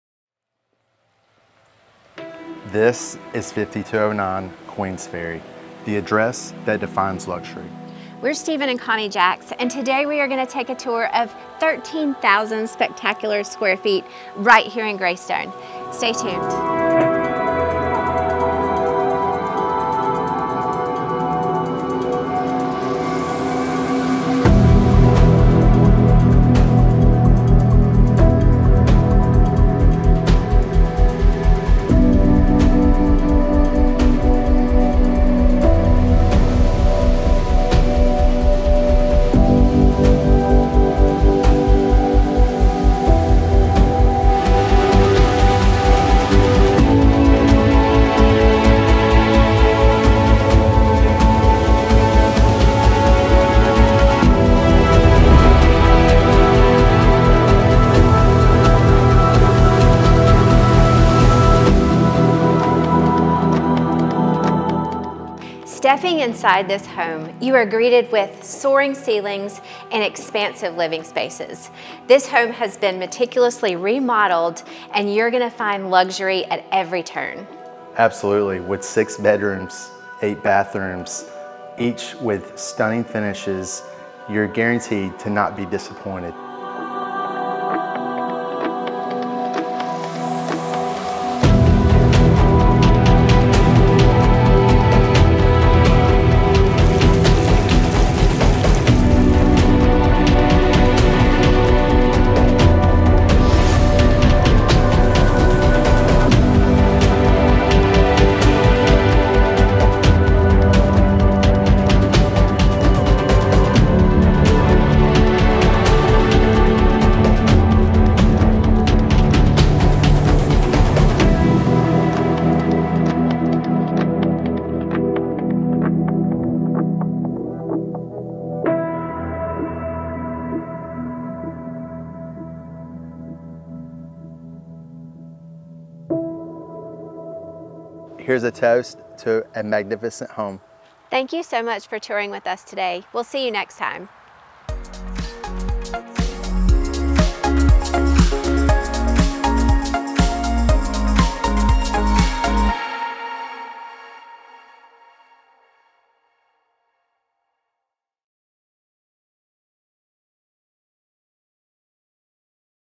5209 Queensferry Lane - Greystone Mansion Home Tour
This is a home tour video I produced at an incredible mansion located in Greystone Country Club, one of the most prestigious communities in the Birmingham area.